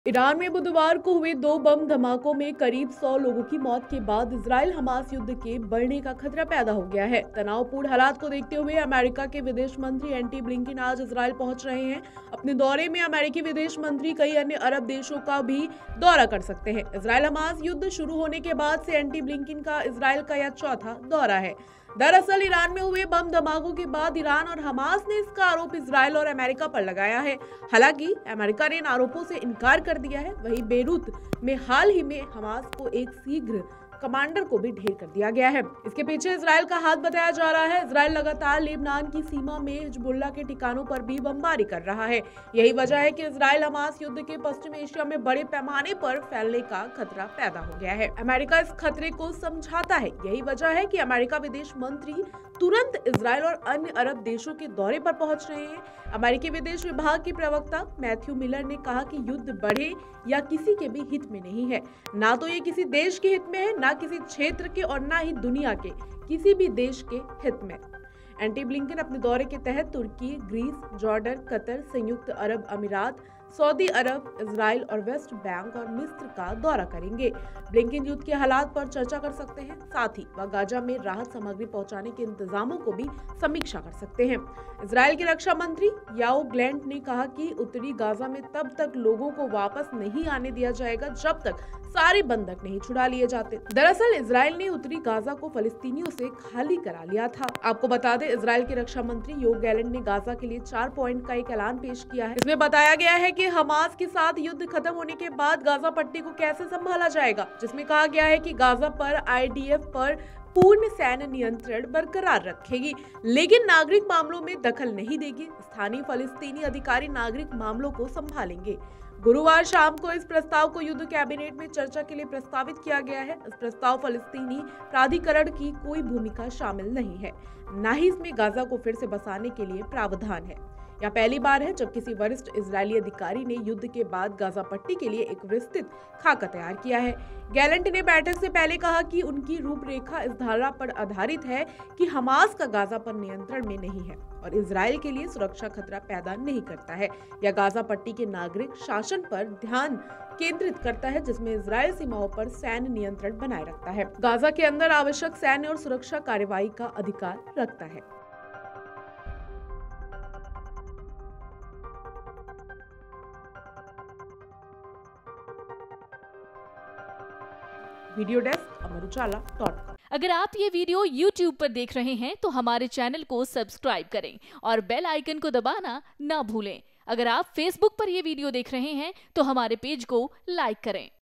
Israel Hamas War: ​Iran बम धमाकों के बाद अब Hamas युद्ध बढ़ने का खतरा! |gaza
ईरान में बुधवार को हुए दो बम धमाकों में करीब 100 लोगों की मौत के बाद इस्राइल हमास युद्ध के बढ़ने का खतरा पैदा हो गया है।